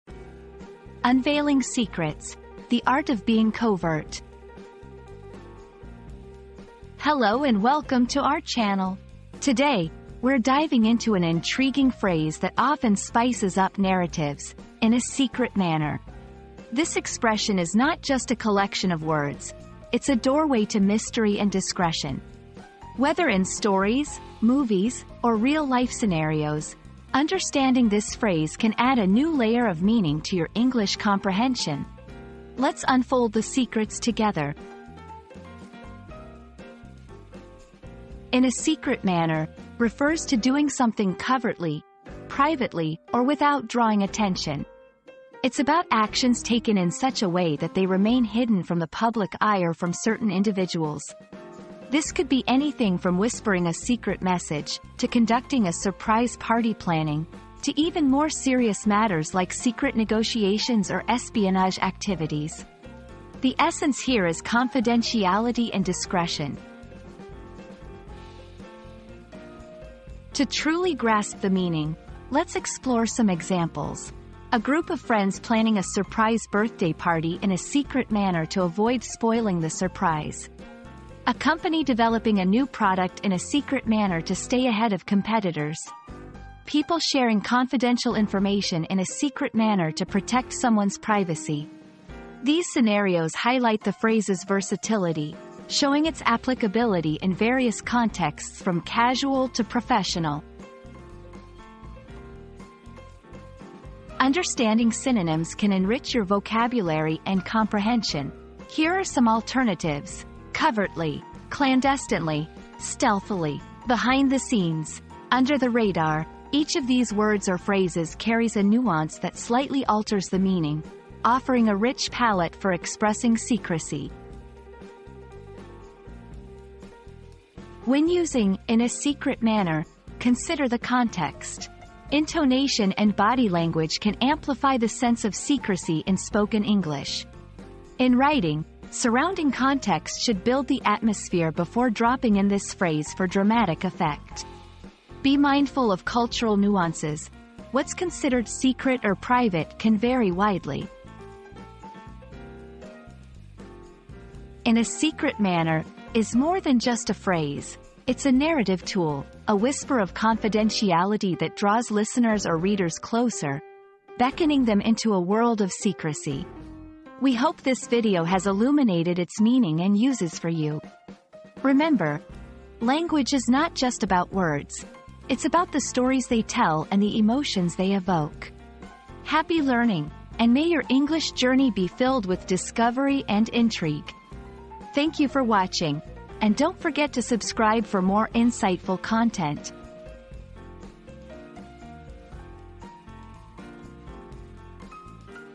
Unveiling Secrets: The Art of Being Covert
00:00 • Introduction - Unveiling Secrets: The Art of Being Covert
00:37 • Understanding "In a Secret Manner"
01:09 • Examples in Everyday Use
01:43 • Synonyms and Related Phrases
02:07 • Practical Tips for Usage